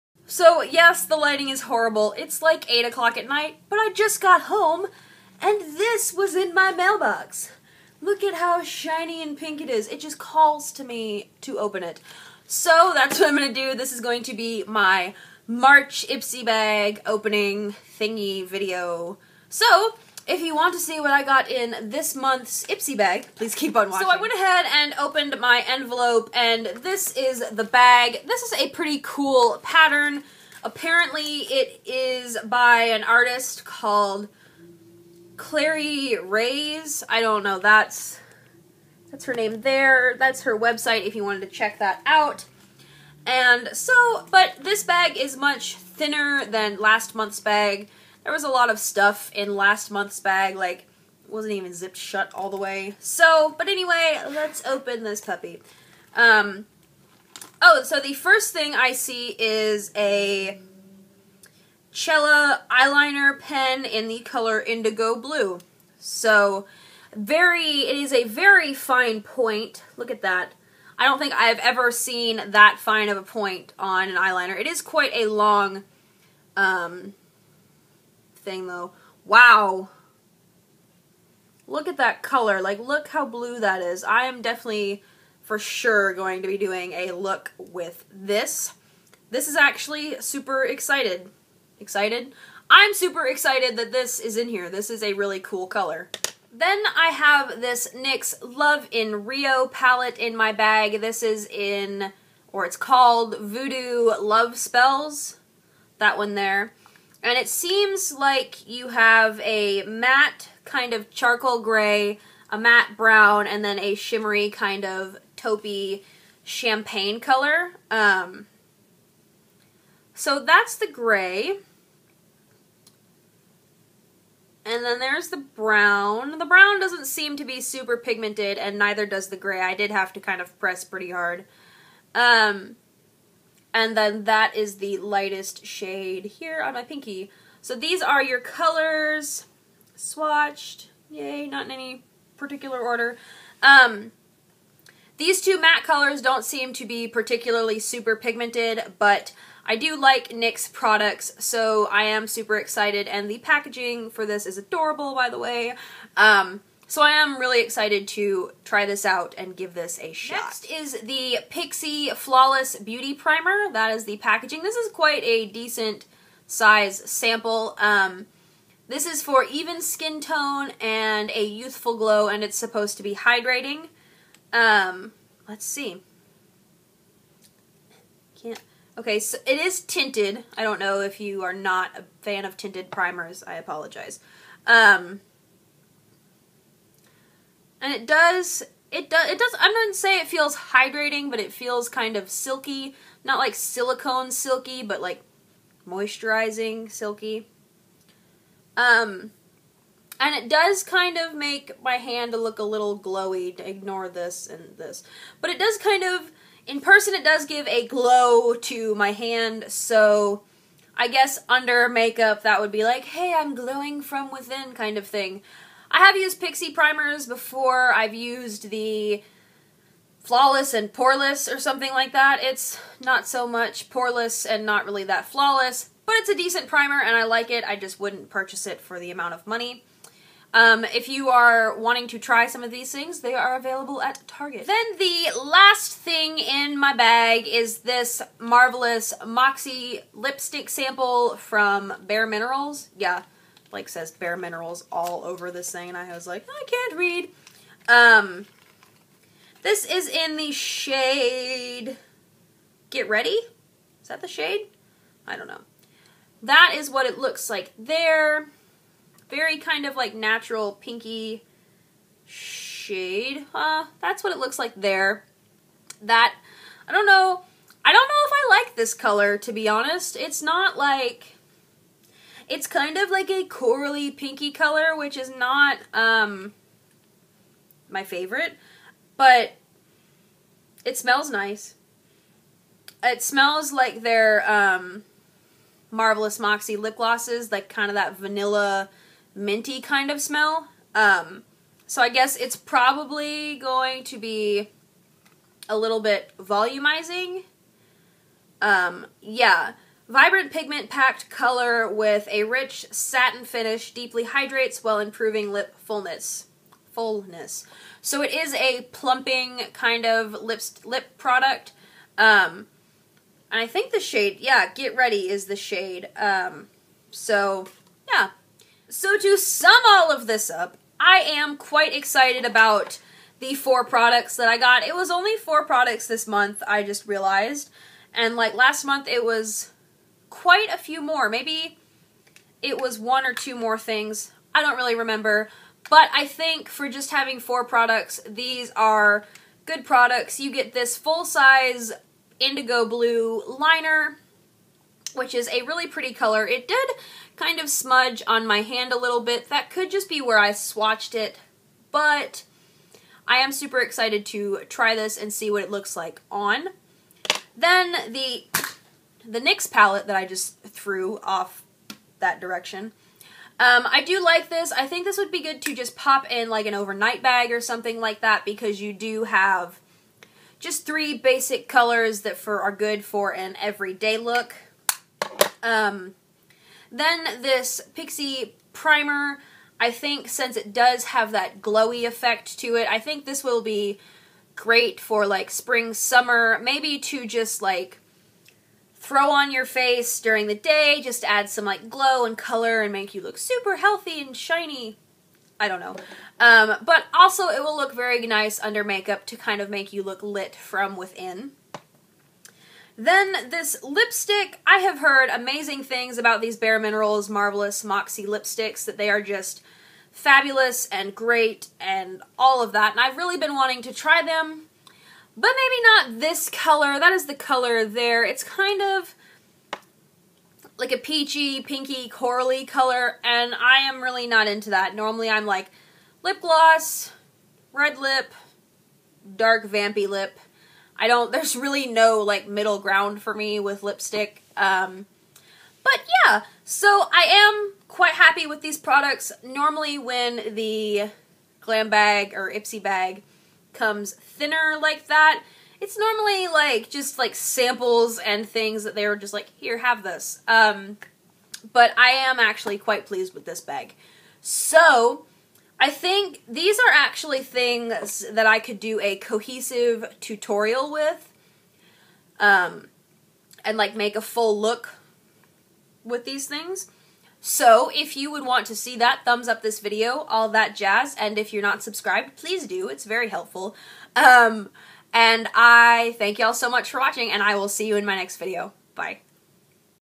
Ipsy Bag Opening March 2014
Not to much of description needed for this I suppose...

But like I said in the video I was a little worried that I was going to hate this bag but I really, actually liked it a lot!